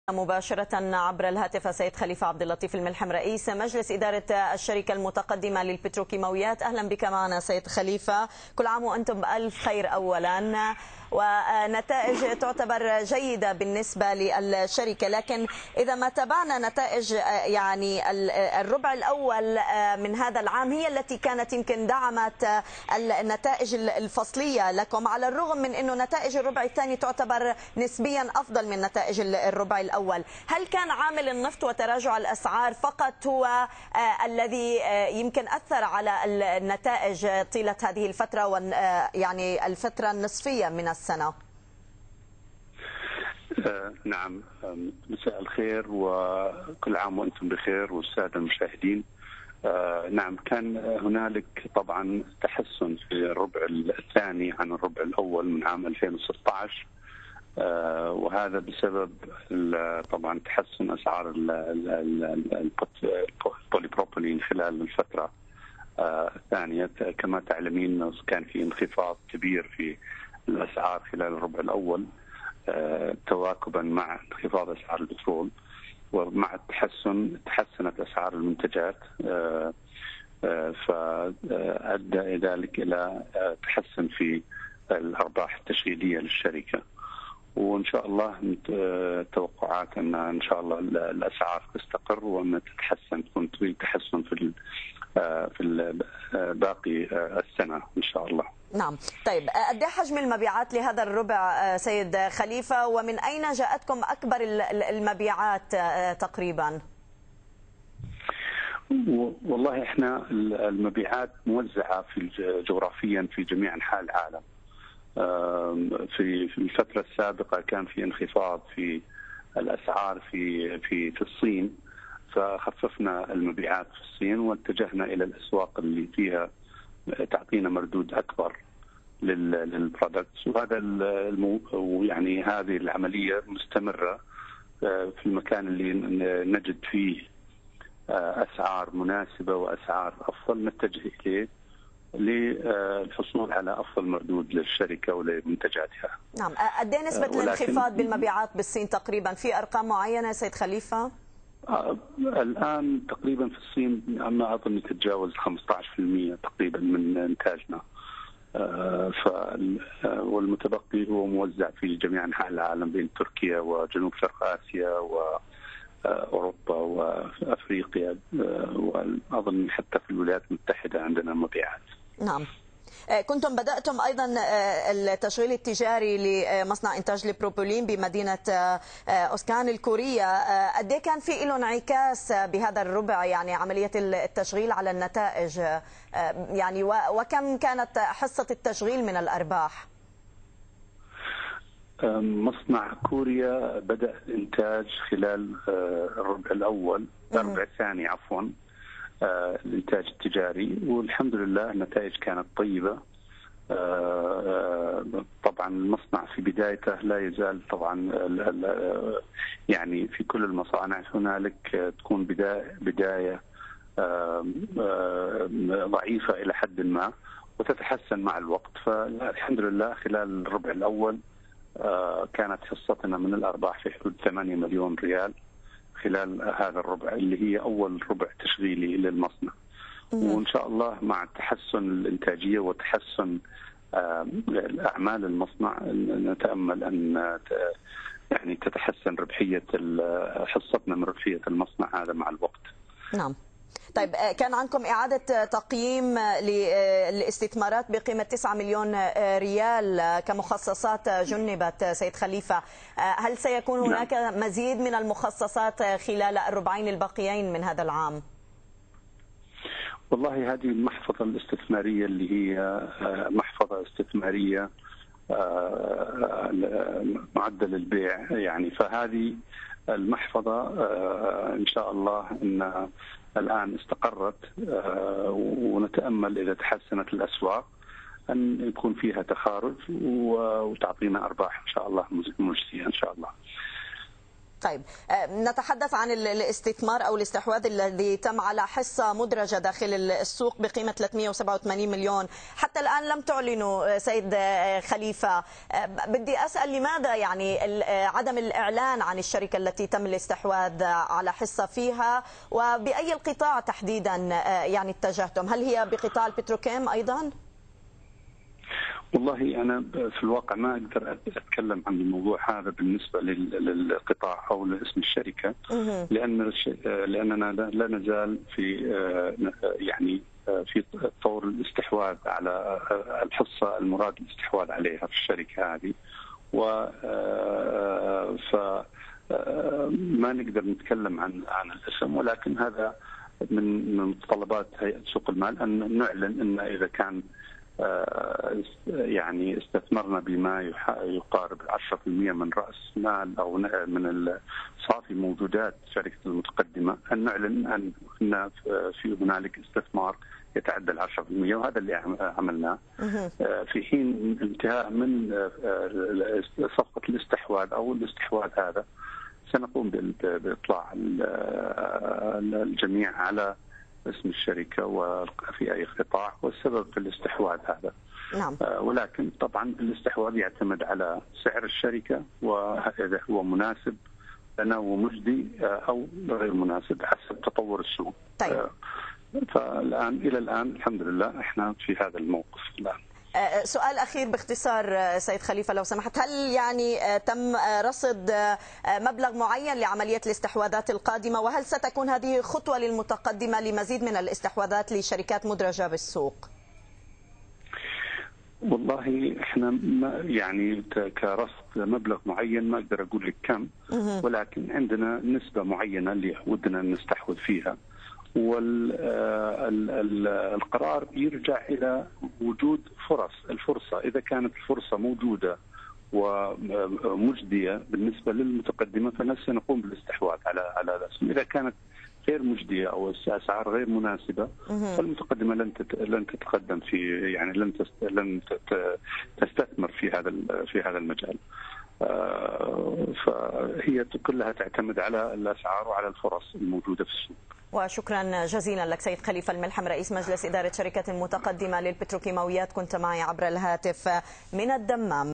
الملحم: مبيعات "المتقدمة" للبتروكيماويات انخفضت 15% في الصين
أكد رئيس مجلس إدارة الشركة المتقدمة للبتروكيماويات السعودية خليفة بن عبداللطيف الملحم على ان مبيعات الشركة انخفضت في الصين بنسبة 15% بسبب انخفاض الاسعار، مضيفا ان الشركة اتجهت الى اسواق اخرى والتي فيها مردود مالي اكبر للشركة كدول جنوب شرق اسيا وتركيا وأمريكا وغيرها.

وقال الملحم ان الشركة لديها النية للاستحواذ على شركات في حال كان هنالك فرص مناسبة في السوق.

وكانت الشركة قد اعلنت عن انخفاض ارابحها الصافية في الربع الثاني من العام الحالي بنسبة 24% الى 186 مليون ريال على اساس سنوي.

كما انخفضت ارباح الشركة خلال النصف الاول من هذا العام بشكل طفيف جدا الى 332 مليون ريال. هذا وارجعت الشركة سبب انخفاض ارباحها خلال الفترة الحالية الى
تراجع ارباحها التشغيلية نتيجة انخفاض اسعار البيع على الرغم من زيادة حجم المبيعات وانخفاض اسعار اللقيم.